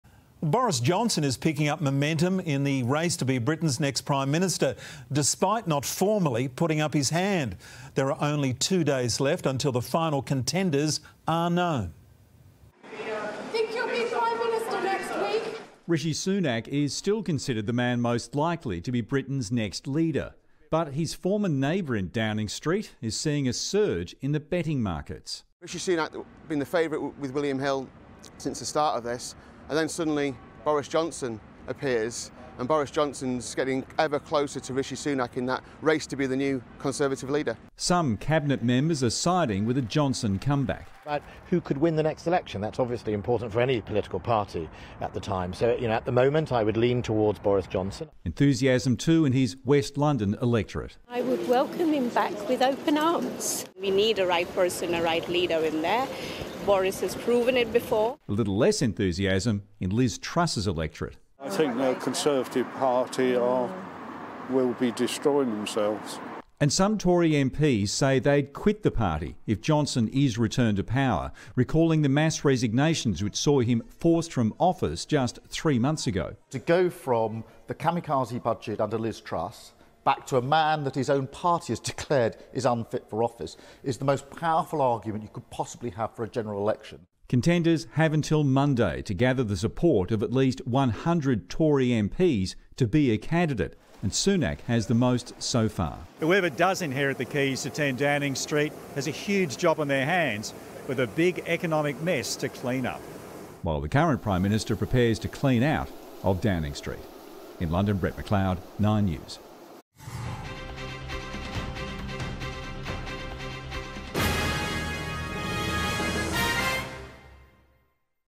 Boris Johnson gains momentum in UK PM race | 9 News Australia
Boris Johnson could make an astonishing return as UK Prime Minister as the jostling for power continues in the wake of Liz Truss' resignation.